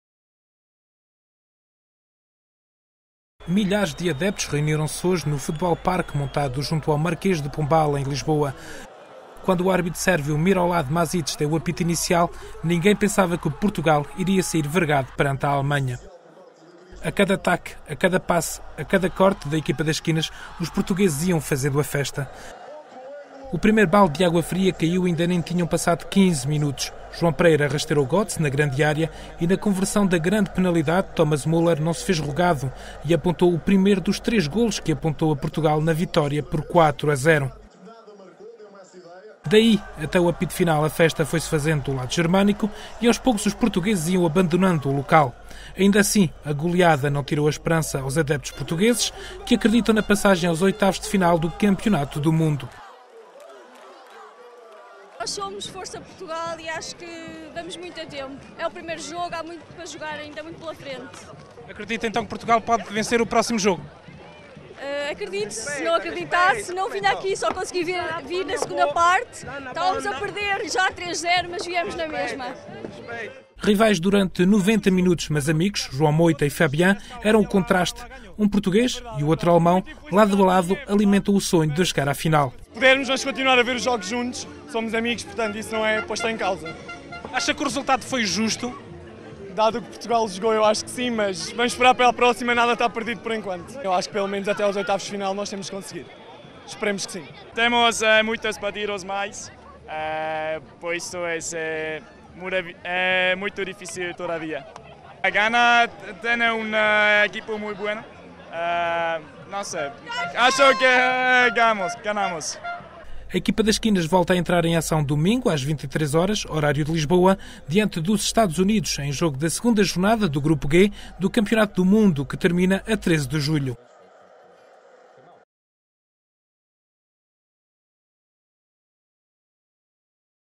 Müller estraga festa no Marquês, mas não esmorece esperança lusa
A goleada sofrida por Portugal frente à Alemanha (4-0), no campeonato do mundo de Futebol, que decorre no Brasil, impediu a festa lusa no Marquês de Pombal, em Lisboa, mas não esmoreceu a esperança na passagem aos "oitavos".

Edição: Carlos Manuel Pereira